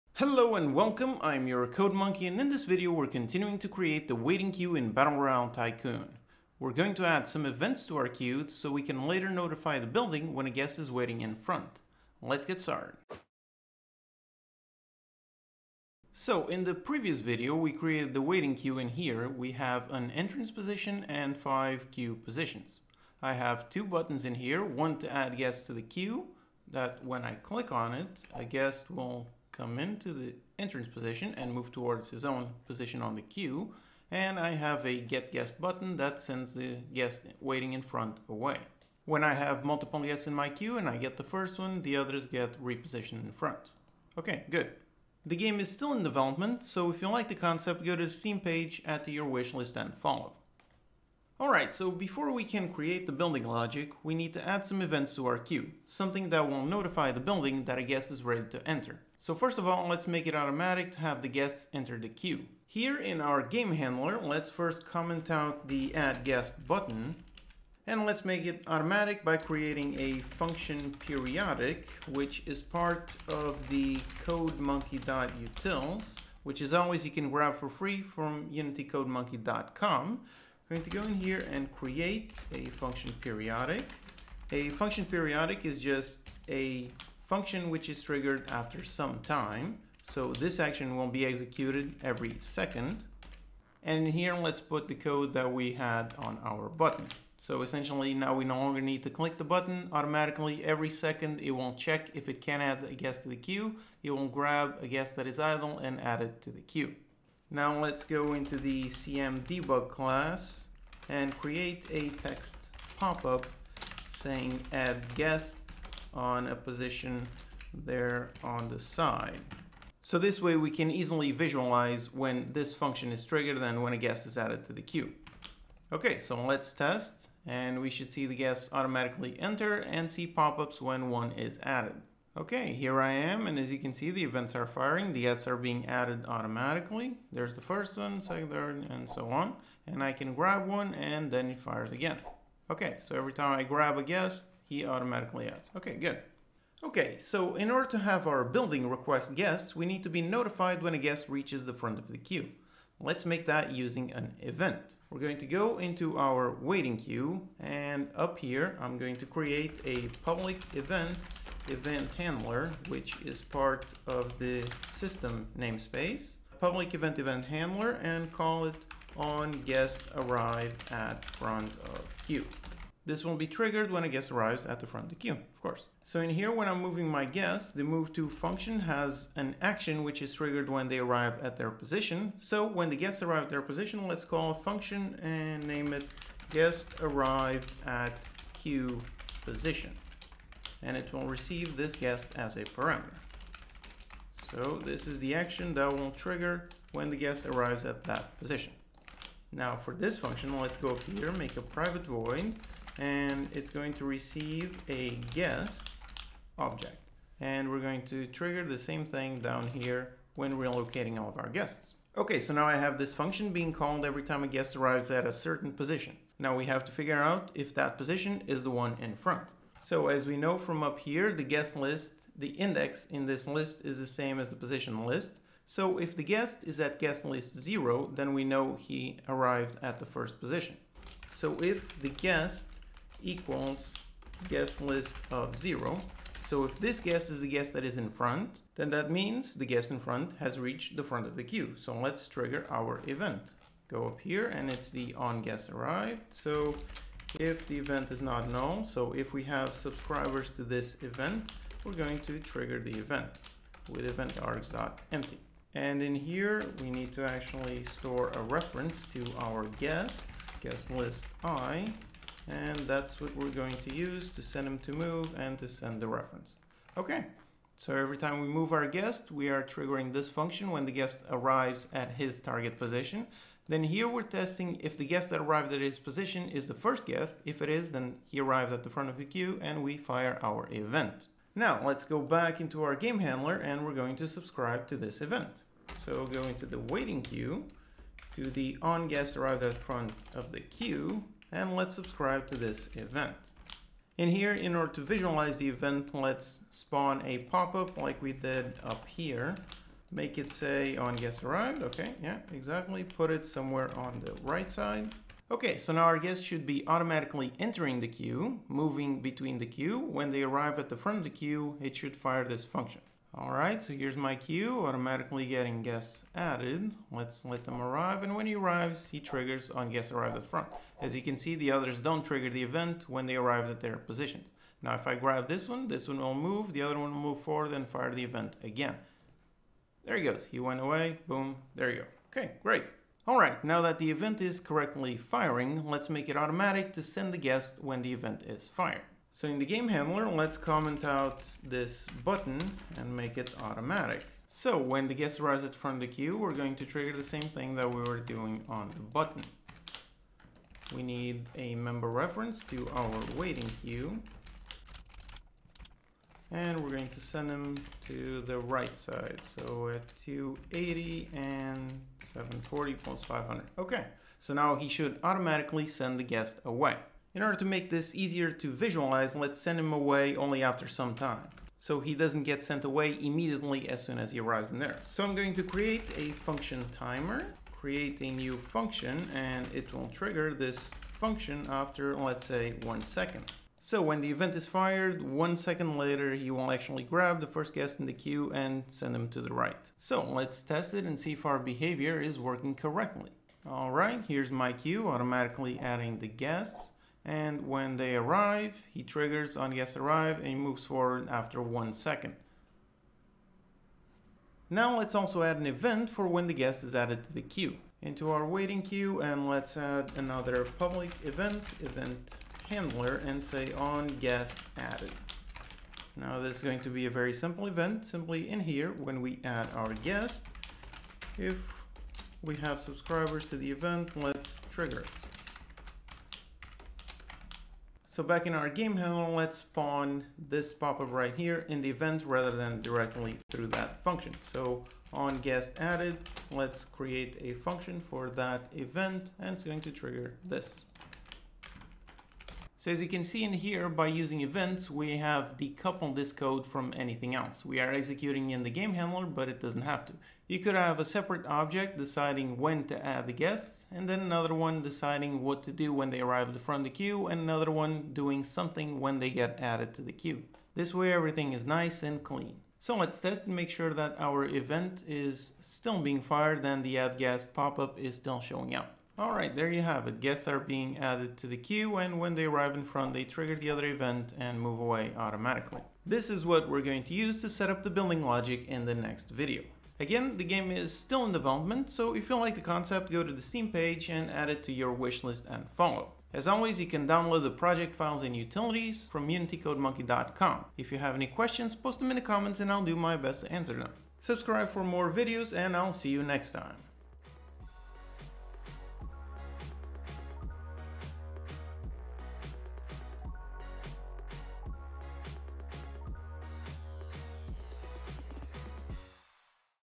Events - Waiting Queue in Battle Royale Tycoon (Unity 2D Tutorial)
Continuing with the Waiting Queue, we're going to add some events so we can later notify the building when a guest is waiting in front.